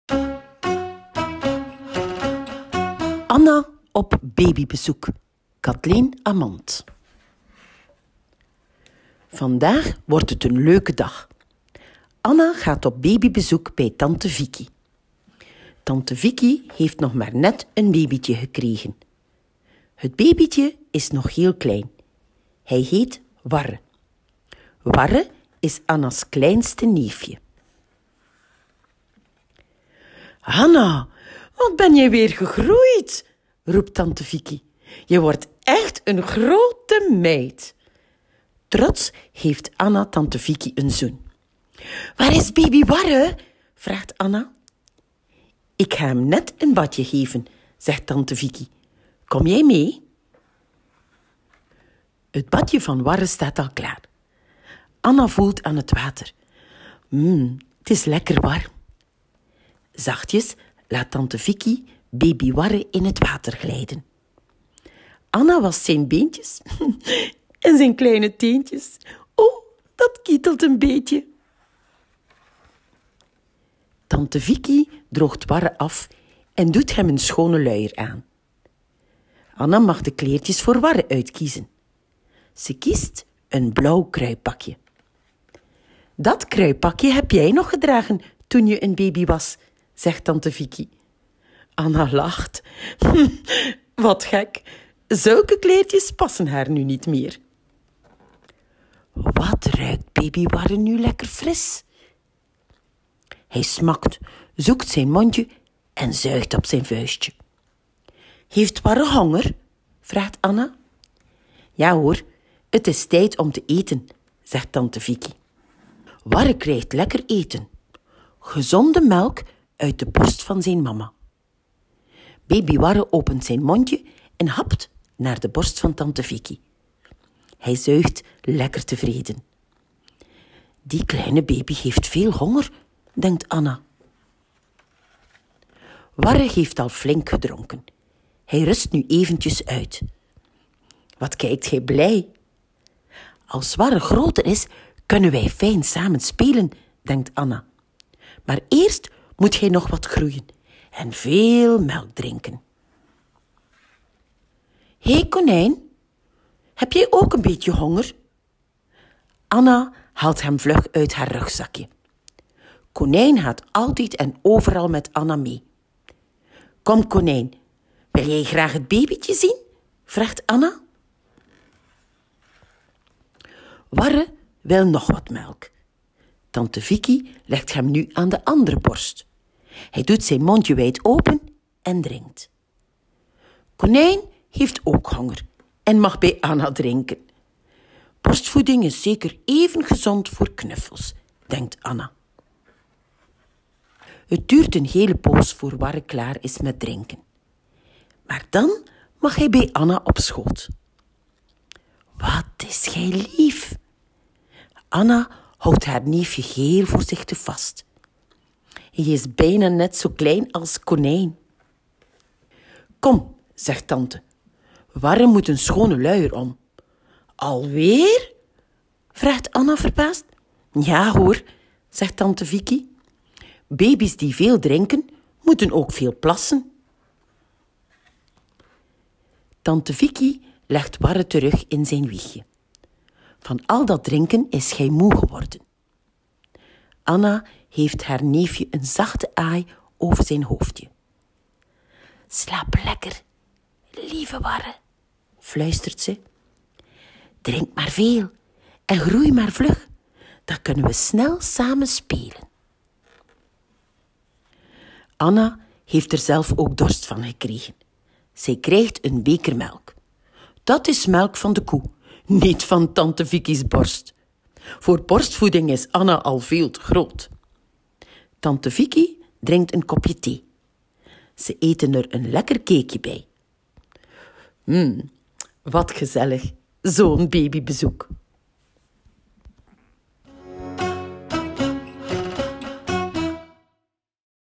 de bib vertelt: Anna op babybezoek ~ Kathleen Amant
Kom je samen met voorleesfee Connie ontdekken hoe Anna kennismaakt met haar neefje, baby Warre?

"Anna gaat vandaag op babybezoek bij tante Vicki. Tante Vicki heeft nog maar net een baby'tje gekregen. Het baby'tje heet Warre en is nog heel klein. Eerst geven Anna en tante Vicki baby Warre samen een badje, dan is het tijd om melk te drinken. Baby Warre drinkt aan de borst van tante Vicki. Dat is moedermelk, gezond en lekker voor baby's."

► uit: Anna op babybezoek ~ Kathleen Amant [Clavis]
► peuterboekje voor kinderen tot drie jaar
► Wil je dit boekje thuis zelf (voor)lezen? Ga dan langs de bib en leen het uit Veel leesplezier!